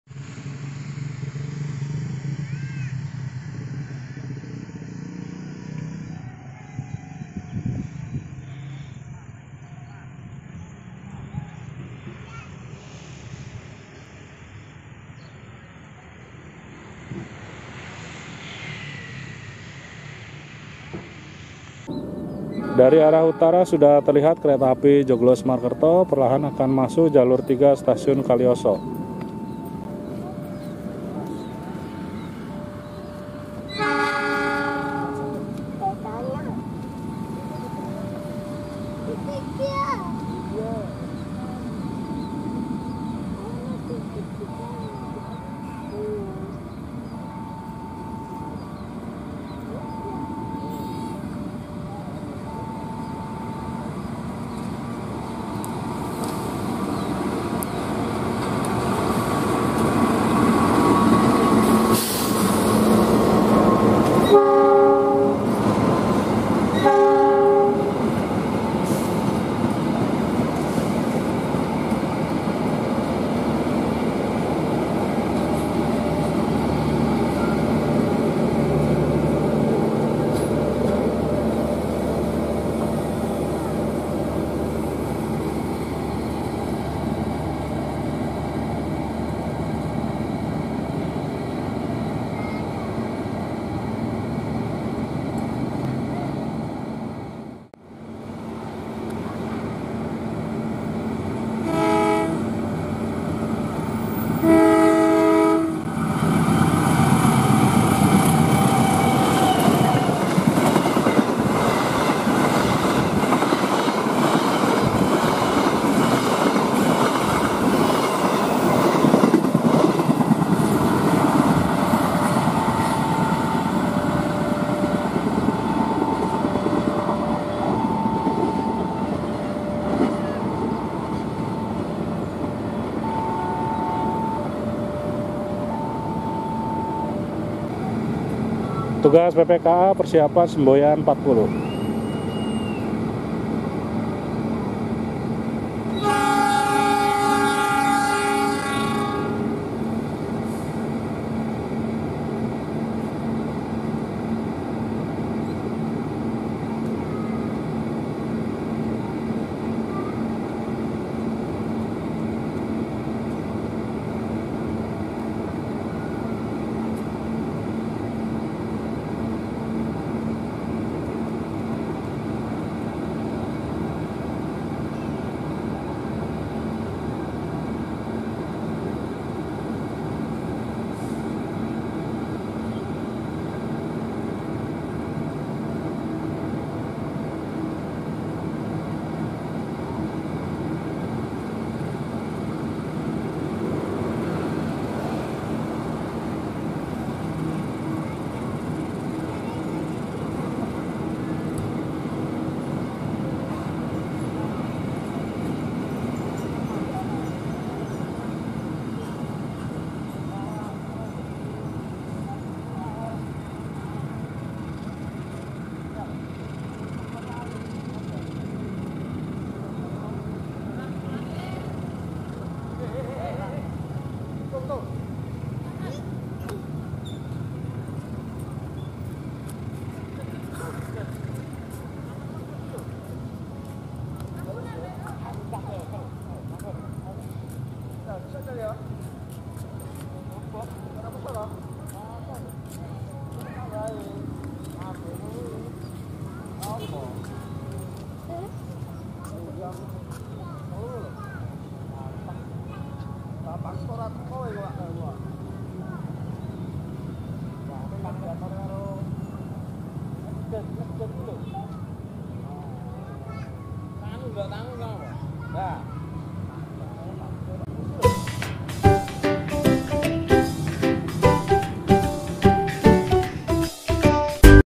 Ngabuburit Nonton Kereta Api Bersilang Di Stasiun Kalioso
Hunting kereta api sore hari sambil menunggu waktu berbuka puasa, saya mencoba mengabadikan momen persilangan antara kereta api joglosemarkerto bersilang dengan kereta api brantas yang akan menuju semarang dan berakhir di stasiun pasar senen..